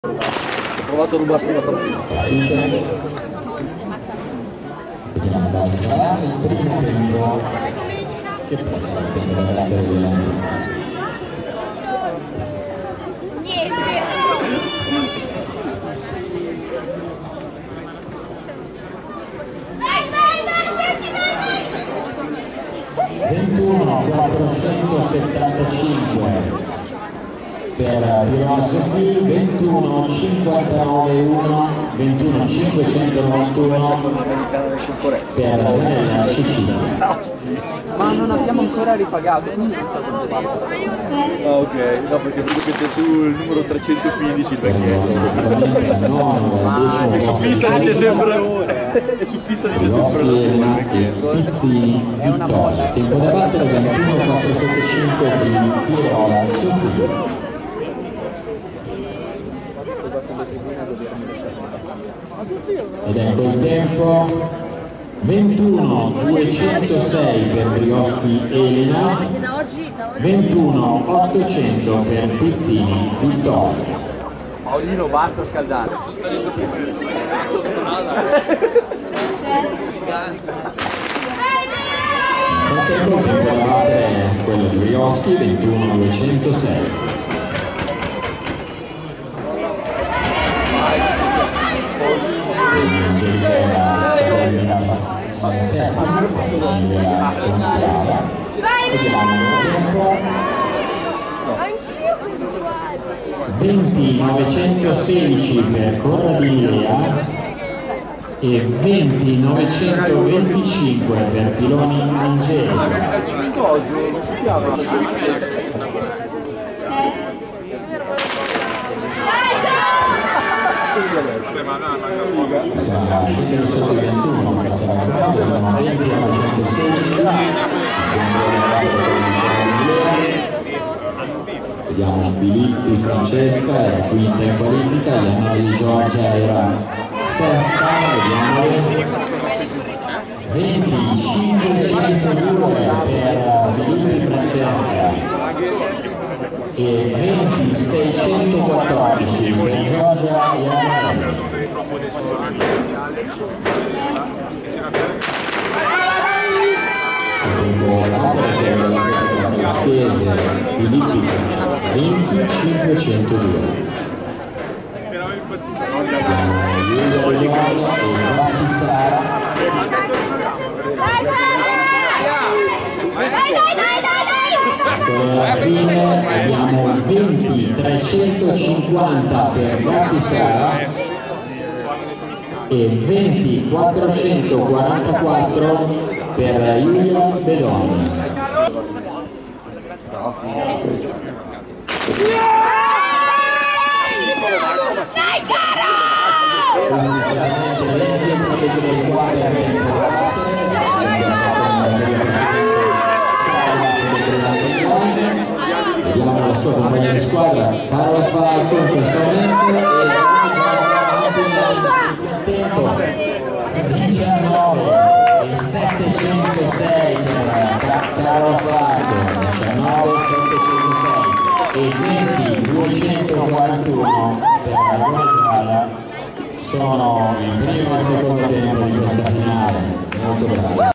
Campionato Regionale Lombardia Pista A-J-S-M 2024 - J/F 1 giro crono ad atleti contr., finale
Campionato Regionale Lombardia A/J/S/M 2024, specialità "pista".
28 Aprile 2024, Bellusco (MB) - Italy.
Organizzazione: A.S.D. Polisportiva Bellusco.
Categoria Juniores Femminile.
1 giro crono ad atleti contrapposti, finale.

00:00 batteria 1
00:36 batteria 2
01:15 batteria 3
01:53 batteria 4
02:31 batteria 5
03:07 batteria 6

1 508 PIROLA SOPHIE ELISA 310 POL. BELLUSCO 21.475
2 498 ZANELLA CECILIA 3370 CARDANO SKATING 21.591

1 46 BRIOSCHI ELENA 310 POL. BELLUSCO 21.206
2 238 PIZZINI VITTORIA 1220 ROTELL.ROSEDA MERATE 21.800

1 76 CORRADINI LEA 310 POL. BELLUSCO 20.916
2 405 PILONI ANGELICA 697 A.S.DIL.PADERNESE 20.925

1 339 FILIPPI FRANCESCA 4185 POL. CALUSCHESE 20.502
2 48 IANNARELLI GIORGIA 310 POL. BELLUSCO 20.614

1 431 GATTI SARA 3809 A.S.D.BRIANZA INLINE 20.350
2 42 BEDON JULIA 310 POL. BELLUSCO 20.444

1 430 FALCO CAROLA 3809 A.S.D.BRIANZA INLINE 19.706
2 319 RAGONE GIADA MUI 3814 ANPINLINE 20.241